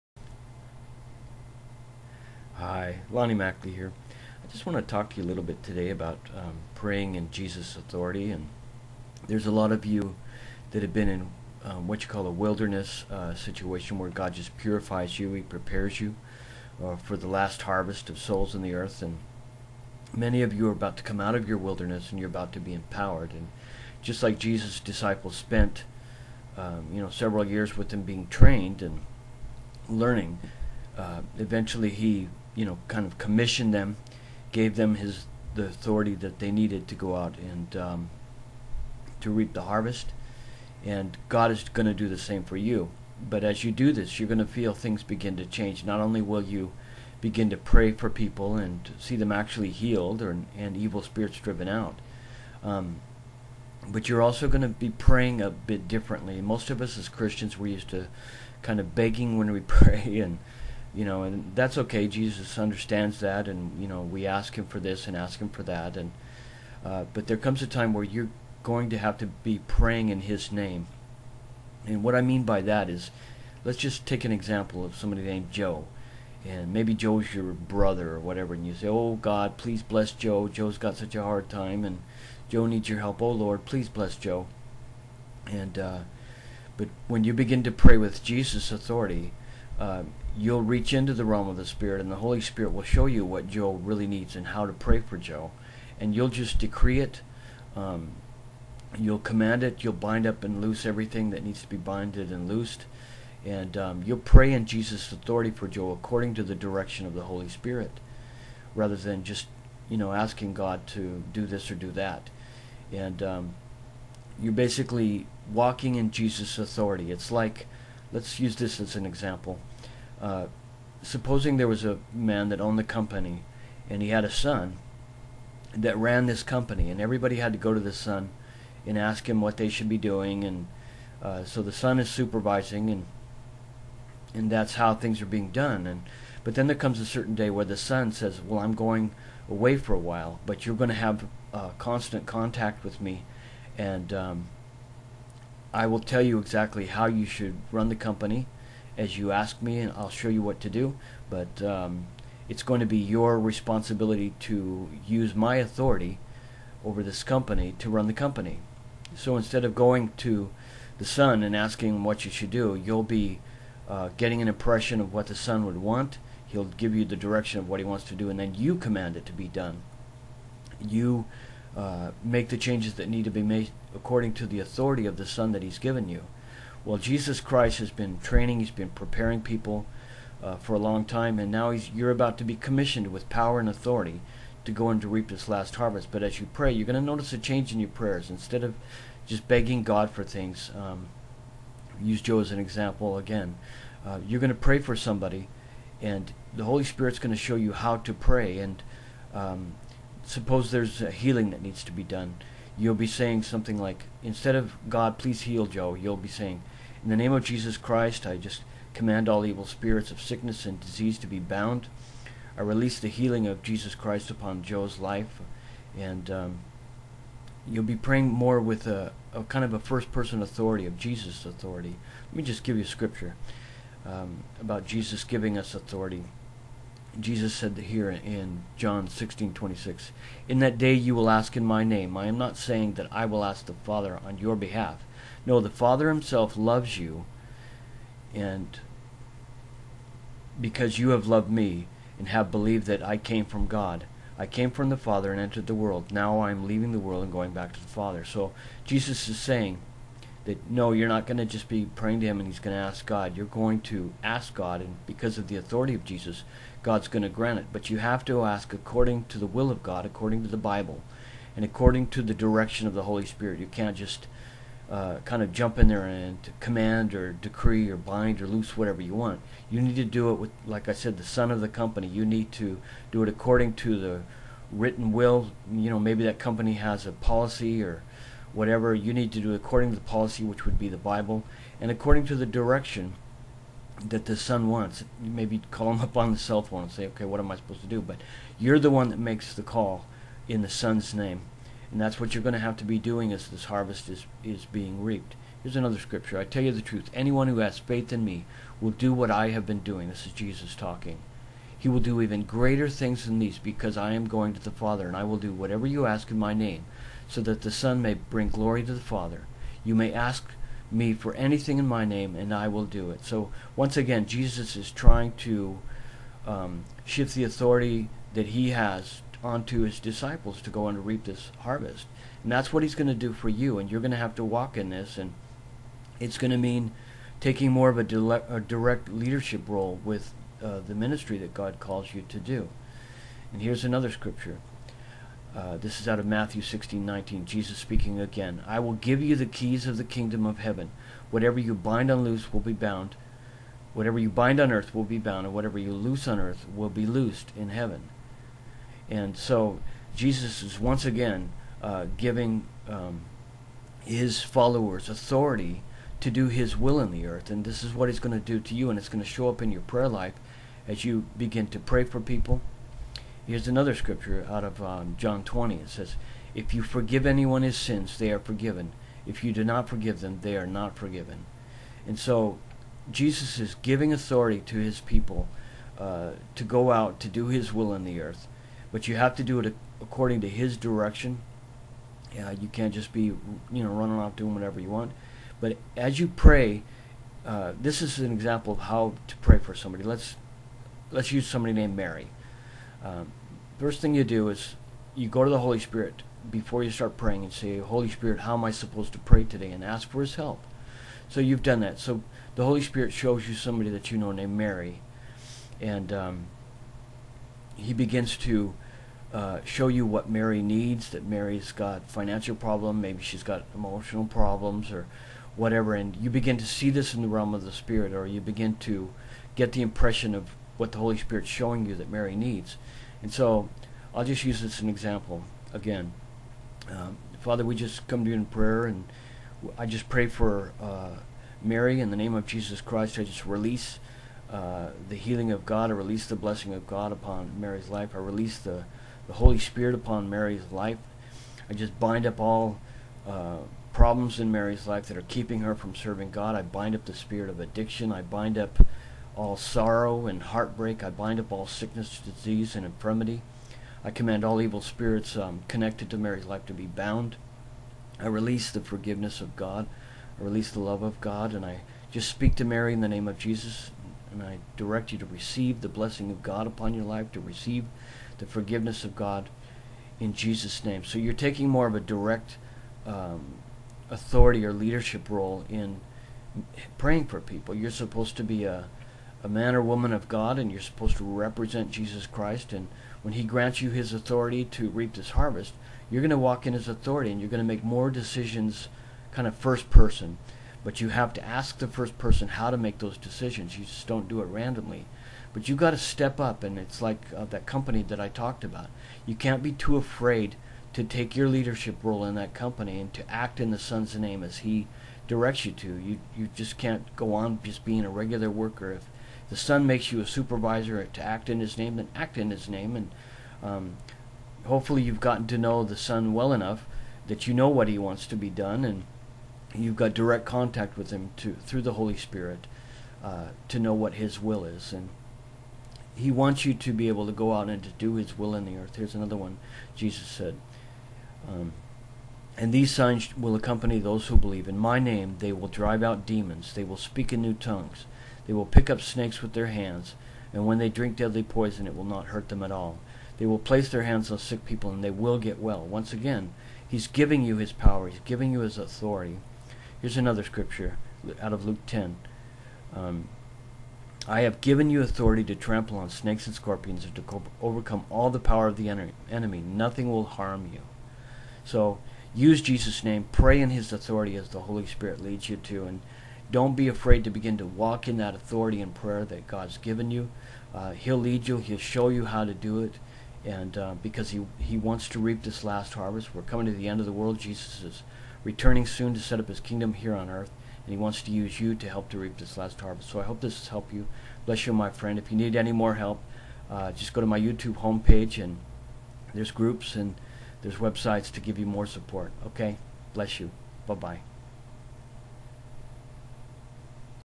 Praying In Jesus Authority by Lonnie Mackley
For many people who have long been in the Christian wilderness you are now about to be released and empowered to reap this last harvest of souls in the earth just before Jesus returns. As this happens you will be given authority to heal the sick, cast out evil spirits, and you will also changes see exciting new changes in how you pray for people with power. Watch this video and see how to pray in Jesus authority.